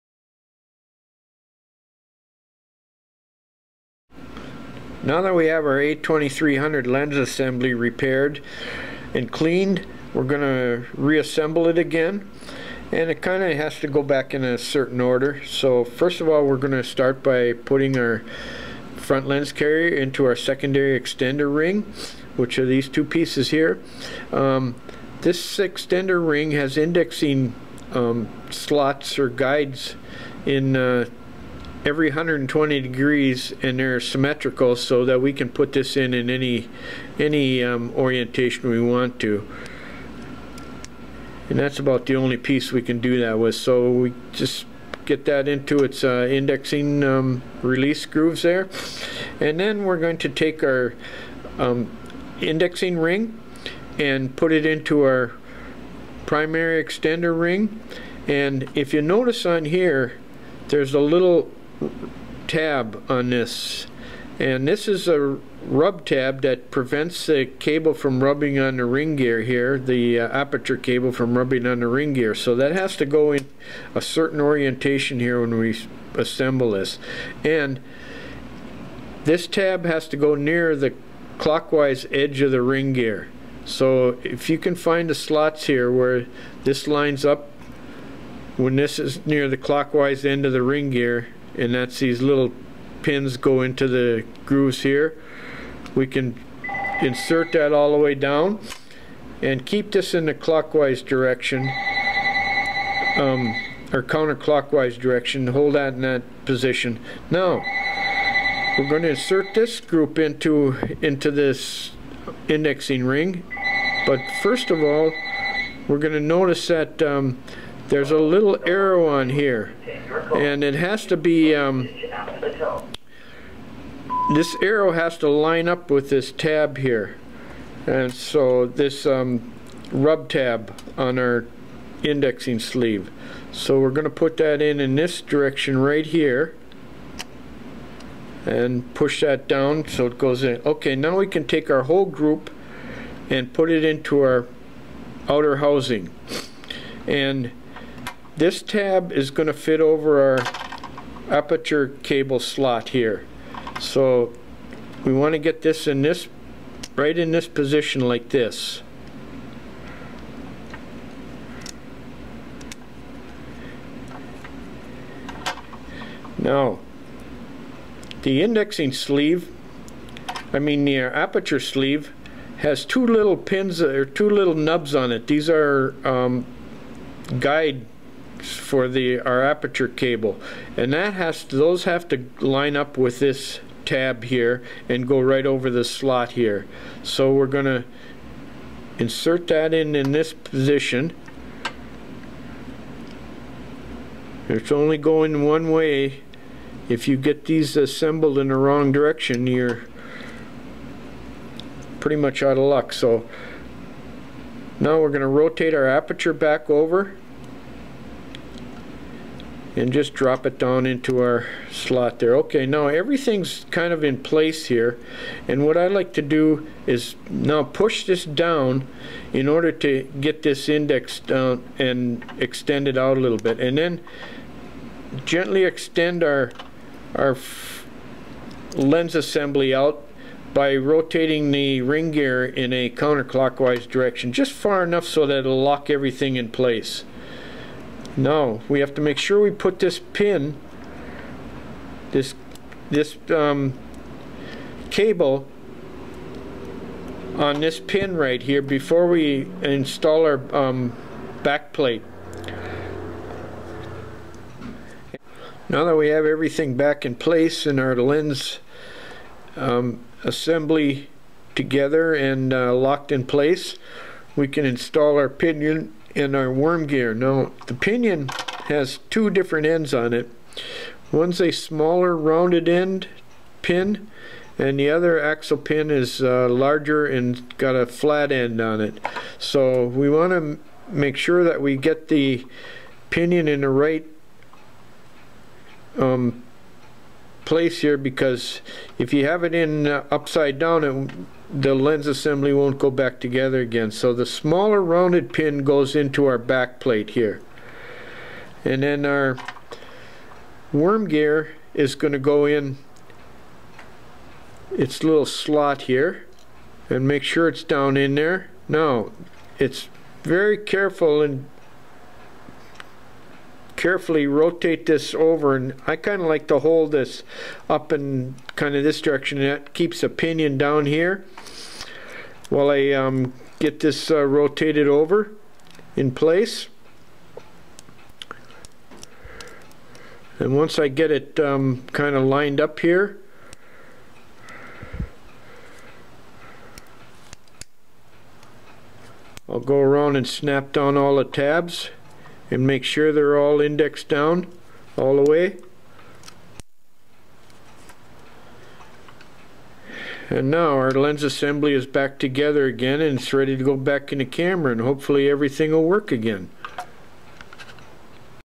Canon A2300 Lens Reassembly
How to reassemble your Canon A2300 lens assembly after cleaning or replacing parts.